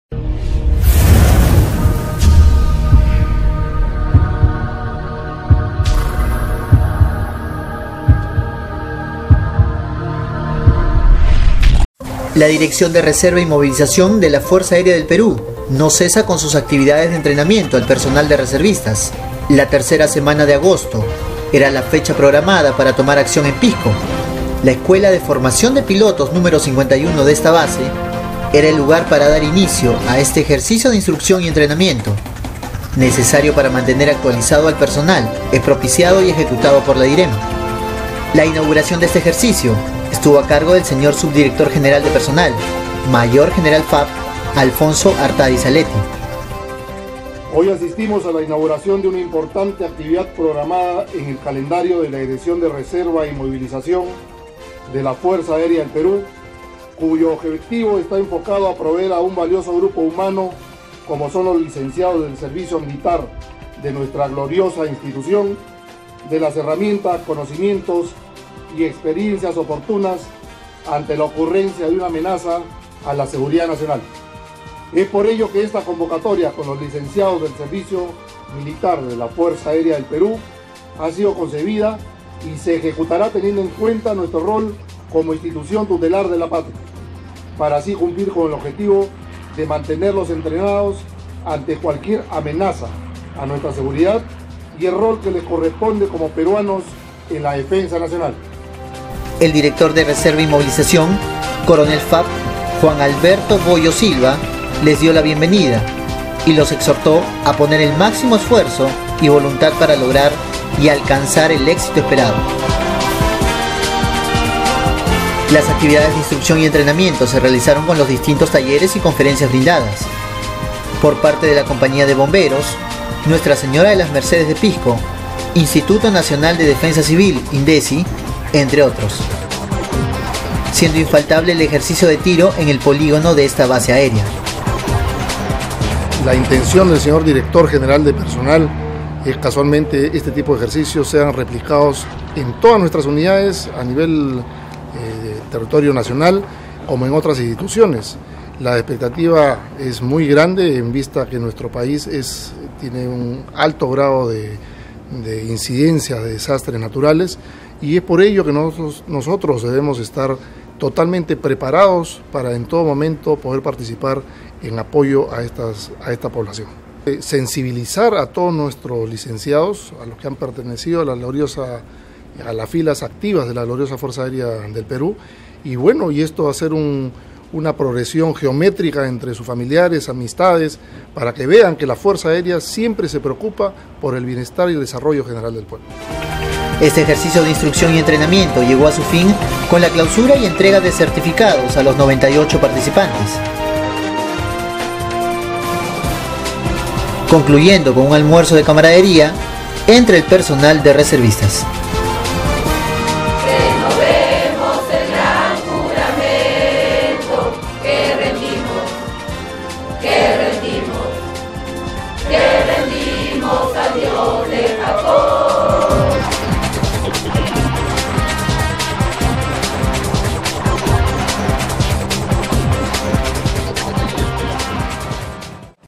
FAPTV: INFORME DE LA RESERVA AÉREA 2015 - PISCO
DIRECCIÓN DE RESERVA Y MOVILIZACIÓN DE LA FUERZA AÉREA DEL PERÚ REALIZÓ LA SEGUNDA FASE DE REENTRENAMIENTO DE LA RESERVA ORGÁNICA DE LA FAP EN LA CIUDAD DE PISCO EN LA  EFOPI -51 PISCO